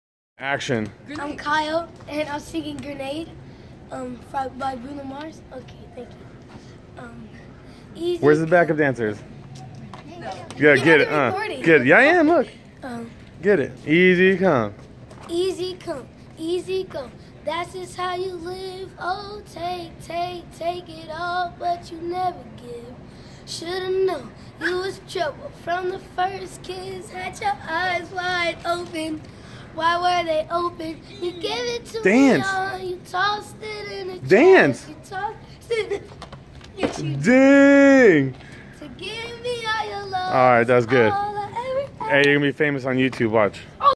America's got Talent 2016
This kid outside of the pasadena convention center begged me to let him in so he could go on stage and perform so I told him he had to impress me first. He didn't cut the mustard lol but I give him props for trying